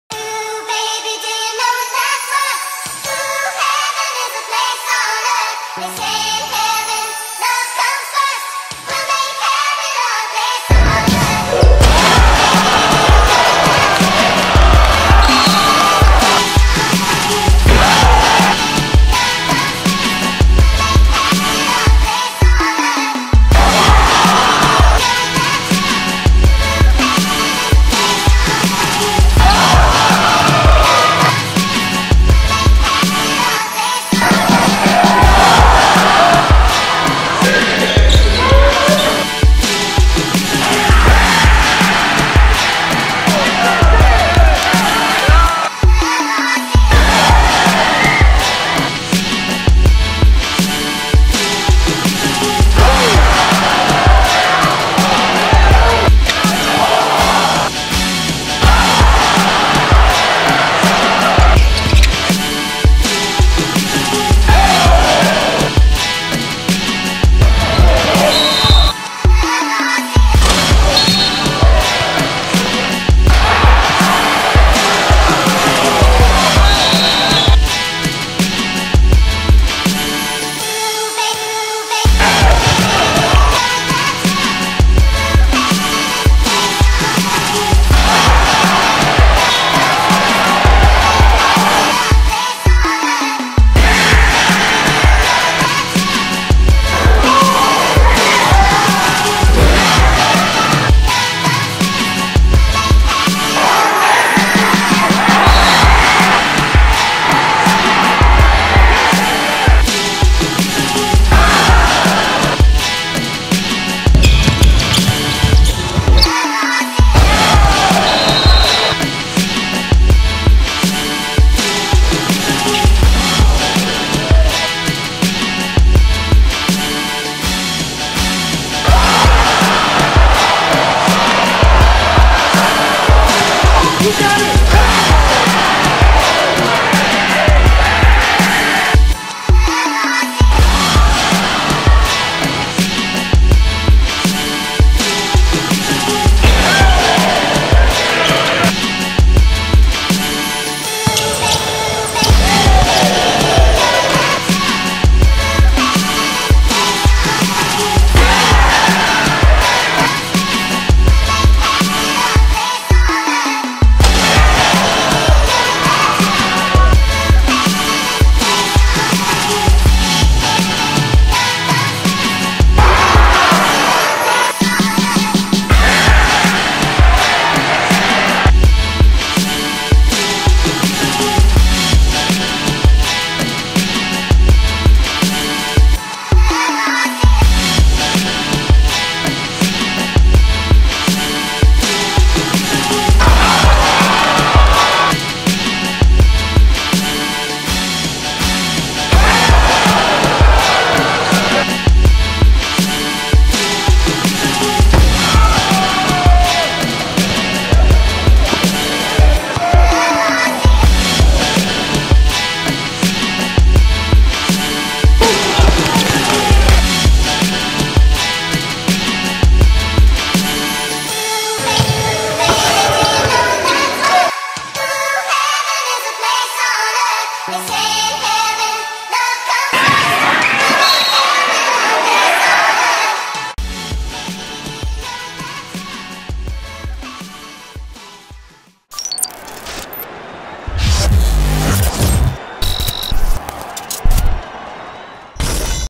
Niven Glover HAS THE MOST BOUNCE IN THE COUNTRY!!! Official Senior Year Mixtape!!!
Niven Glover Senior Year Mixtape!

Fimed/edited by George Mendoza (Elite Mixtapes FL).